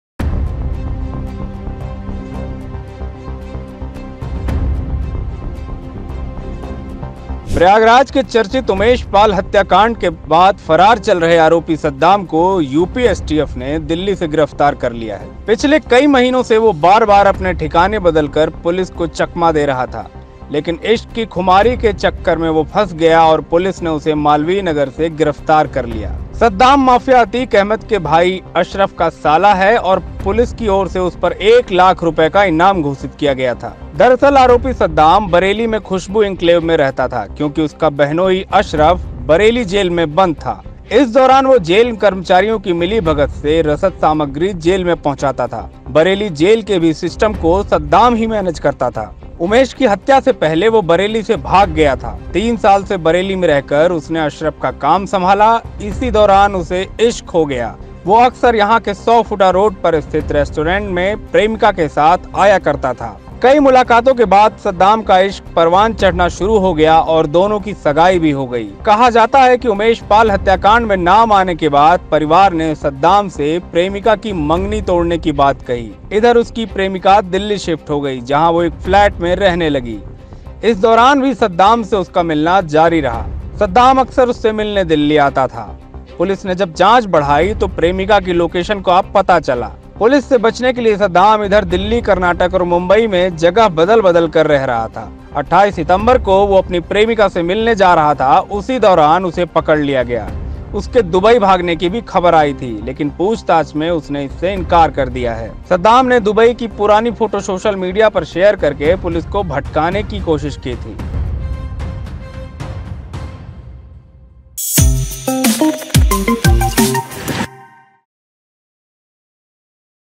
UP News: Atique Ahmed का साला गिफ्तार.STF ने जाल बिछाकर Delhi से दबोचा | ABP LIVE
UP News: Atique Ahmed का साला गिफ्तार.STF ने जाल बिछाकर दिल्ली से दबोचा | ABPLIVE